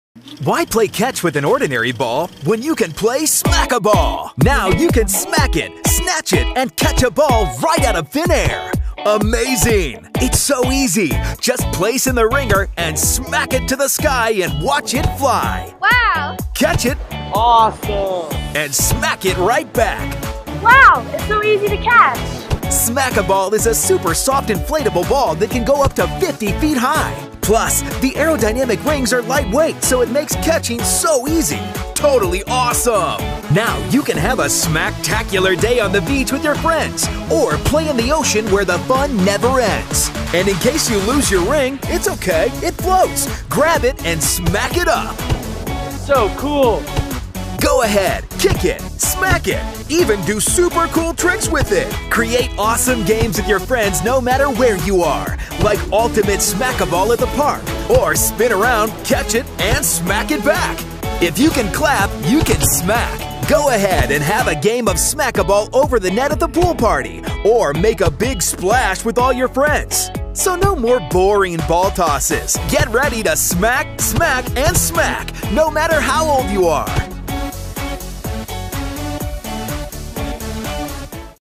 Smak-A-Ball
Supercool buitenspel van Schildkrot Fun Sports. Kom uit die luie stoel en spelen maar. Alleen of met je vrienden. Have fun in the sun. Verkrijgbaar bij voor 24,95